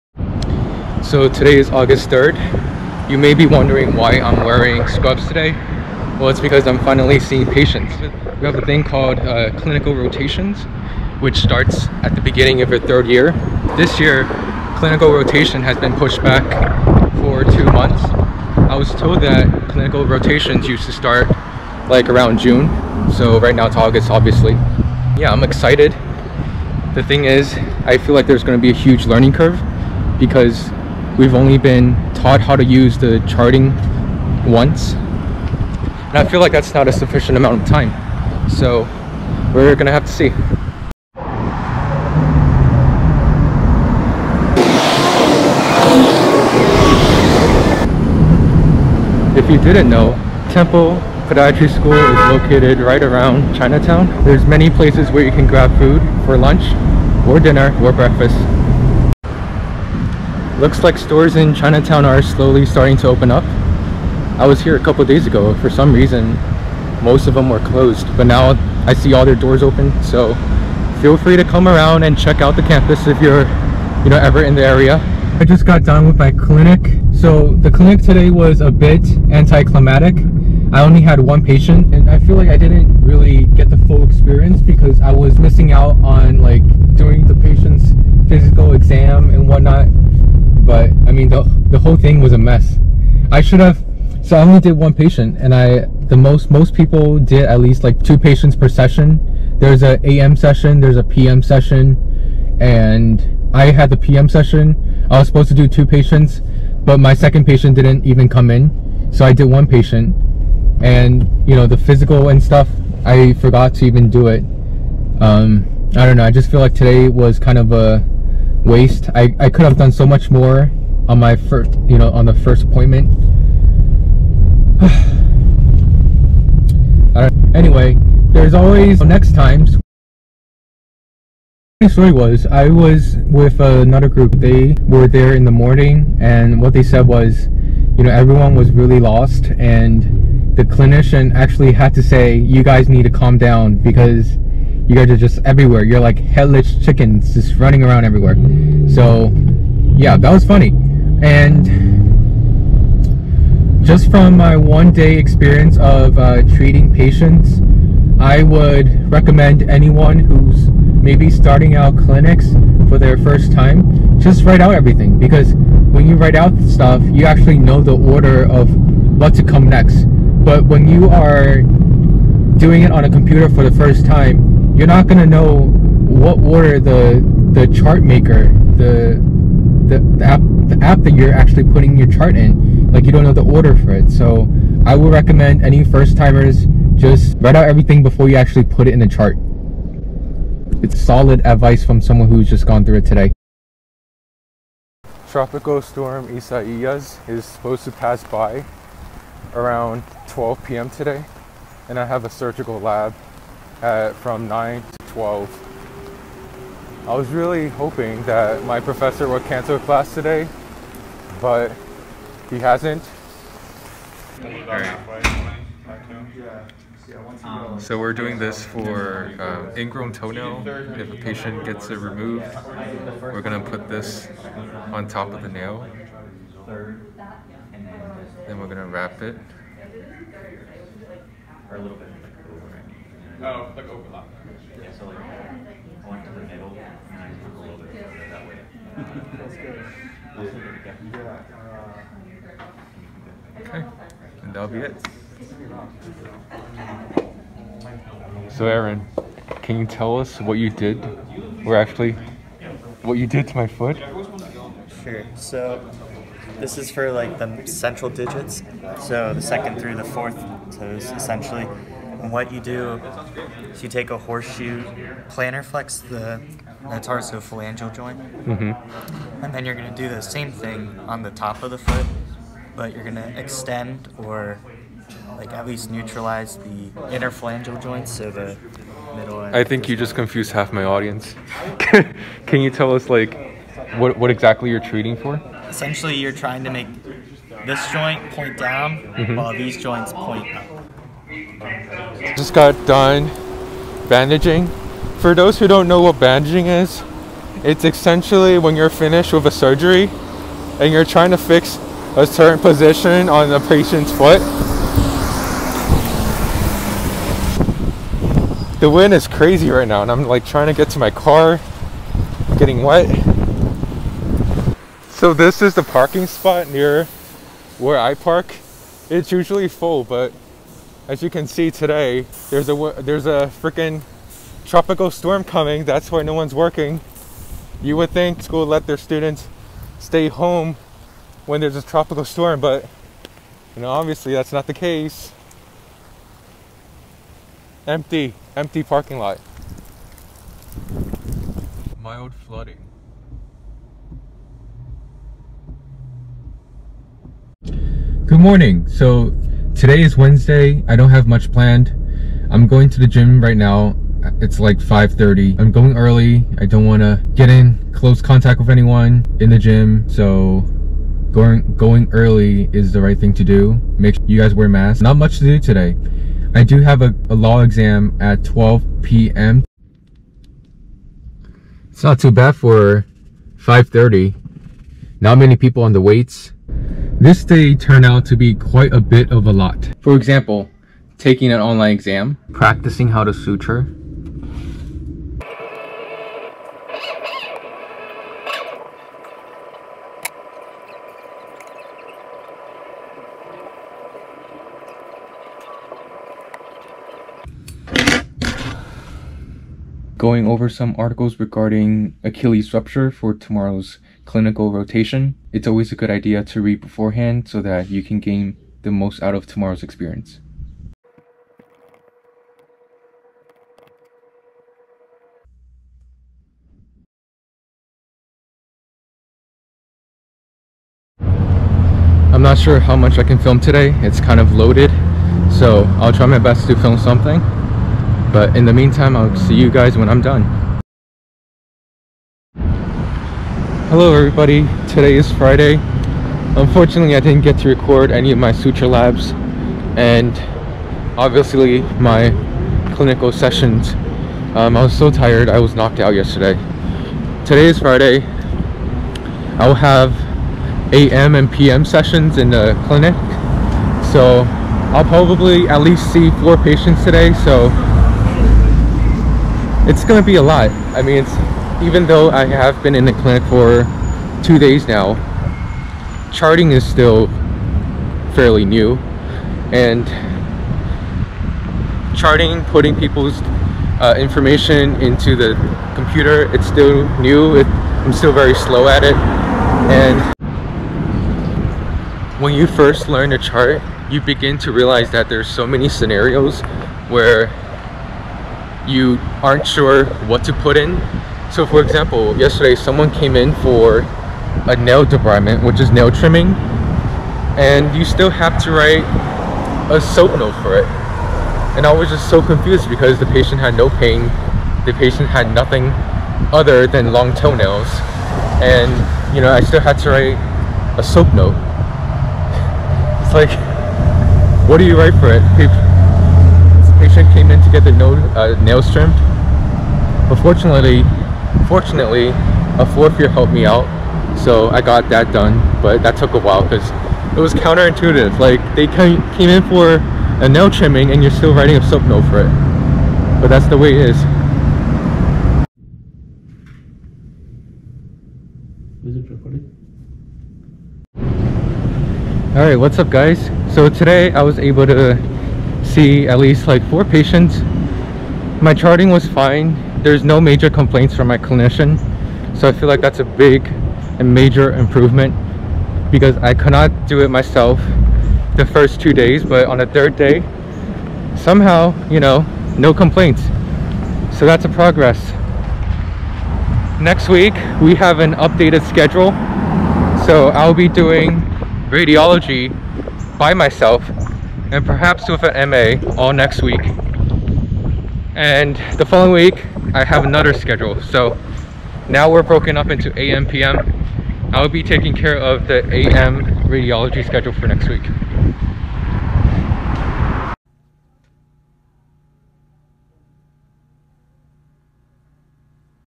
CLINICAL ROTATIONS in Podiatric Medical School
1st week of clinical rotations with social distancing